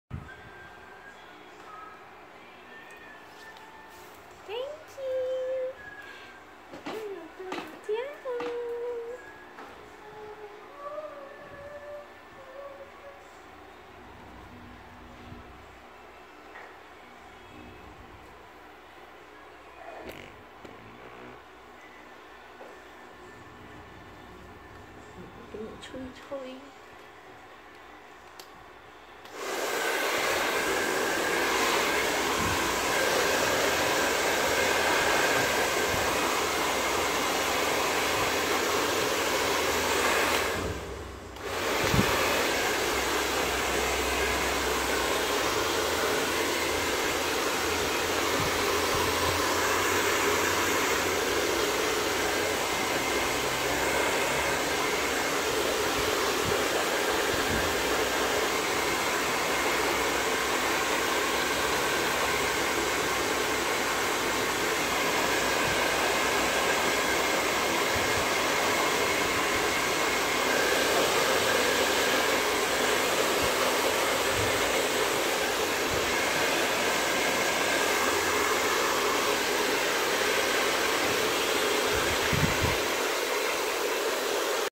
Drying after bath part 1
My baby Miniature Schnauzer. Name Lao Jia Huo.